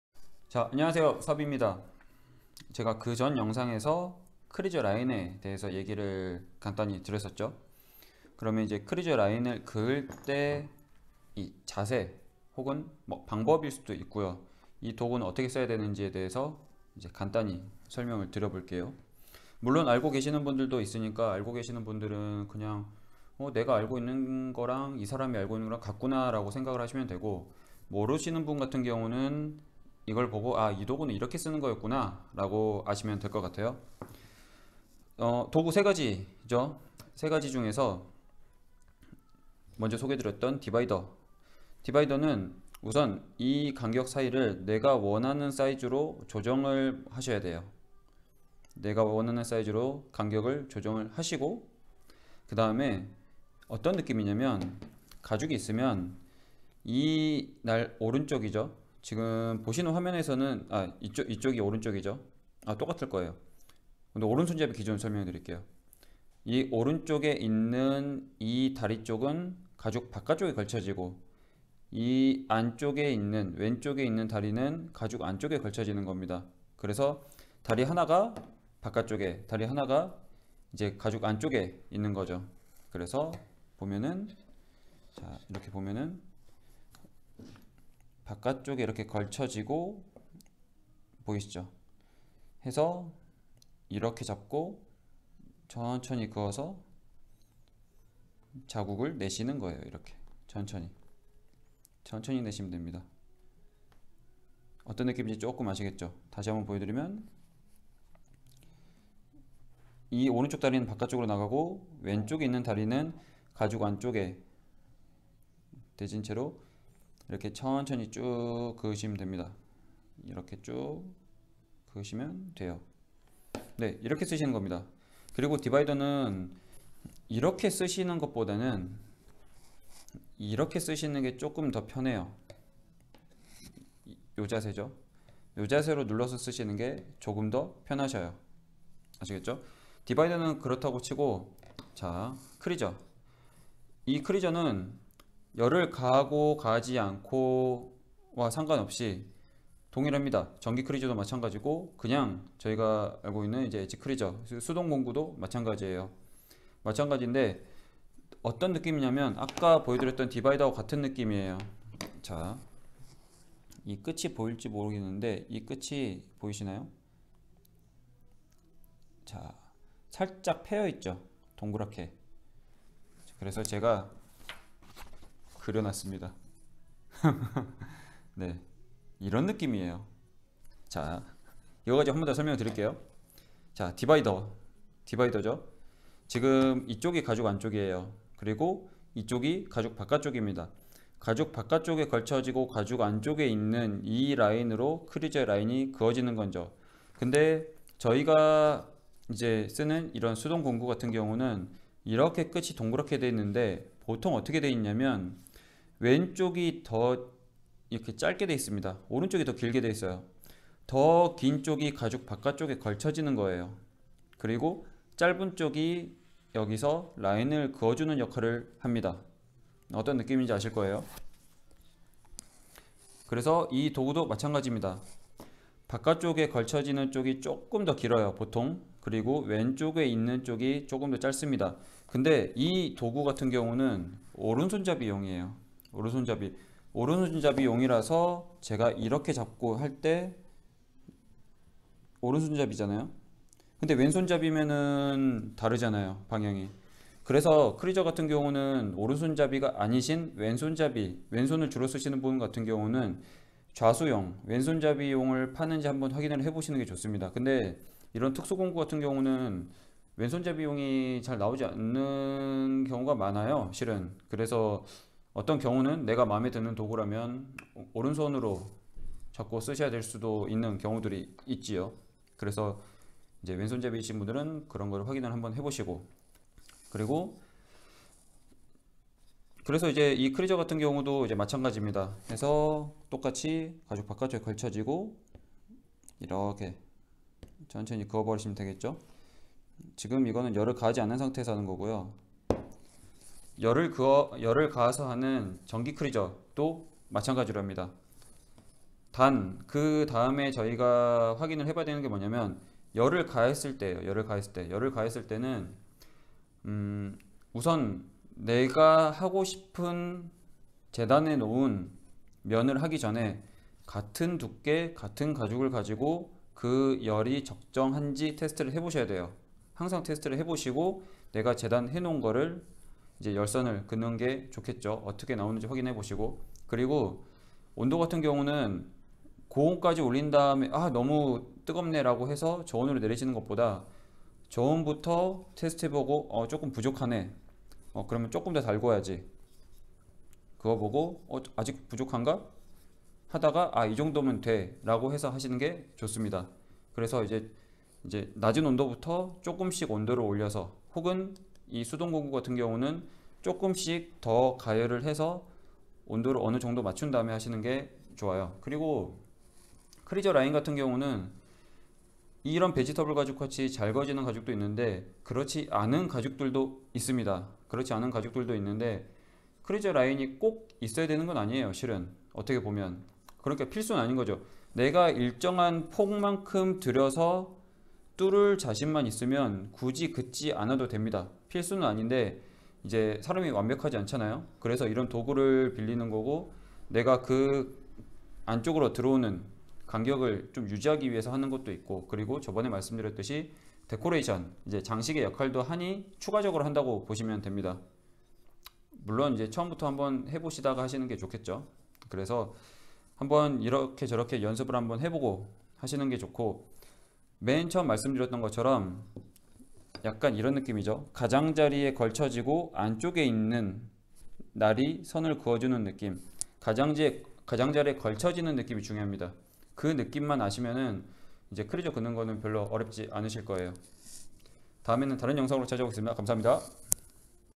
[가죽공예/스킬,테크닉] 크리져선긋기 느낌학개론
* English subtitles have been uploaded.

디바이더, 크리져 등을 이용해서 크리져 선을 긋는 방법을 수치에 기반한 이론이 아닌, 느낌에 기반한 공감대형성기법으로 알려드립니다.